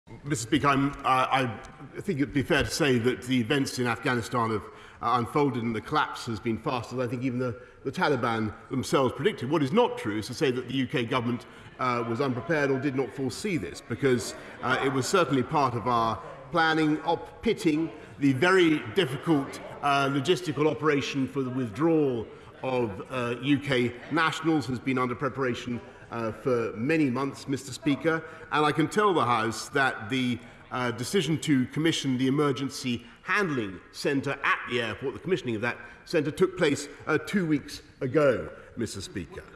UK government ‘foresaw’ Afghanistan crisis, Boris Johnson says
The UK will judge the Taliban regime on its “actions, rather than by its words” after its promises of an amnesty and to protect women’s rights, Boris Johnson has said.

Opening an emergency debate, the prime minister faced criticism from MPs for the chaotic withdrawal from Kabul – and for being on holiday at the weekend, when the capital fell.

But he insisted the UK did “forsee” the Taliban takeover, although the speed of it had come as a surprise.